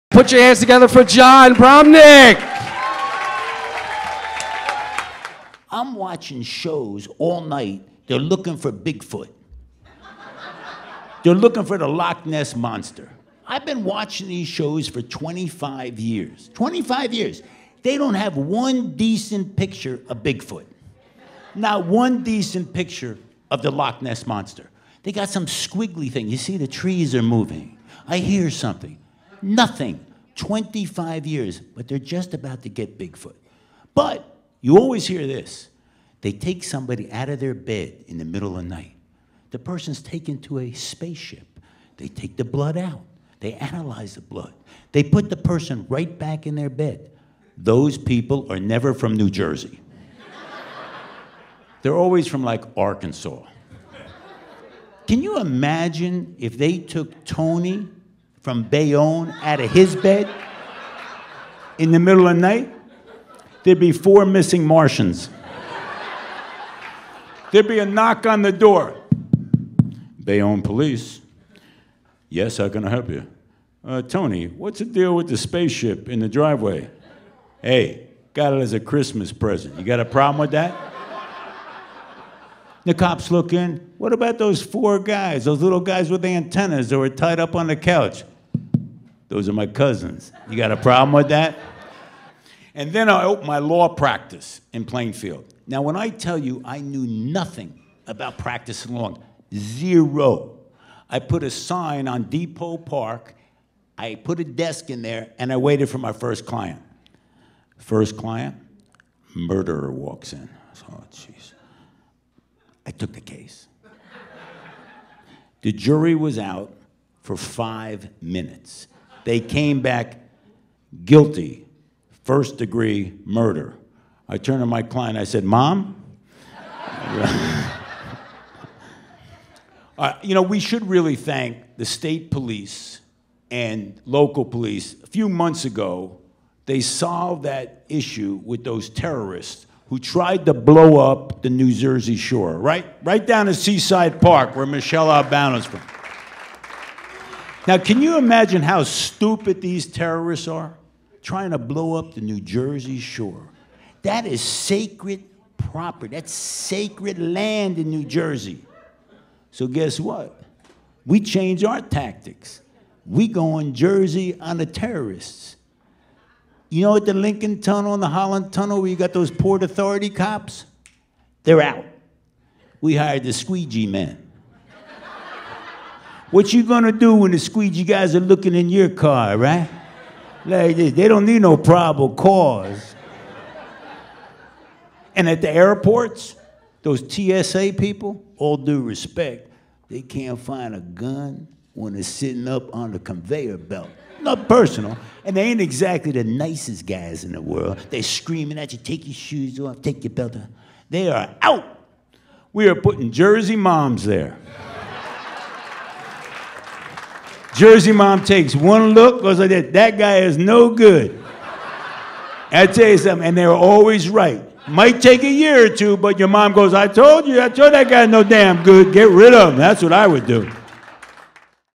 Bramnick at the Stress Factory March 2017
Jon Bramnick performing at the Stress Factory in March 2017.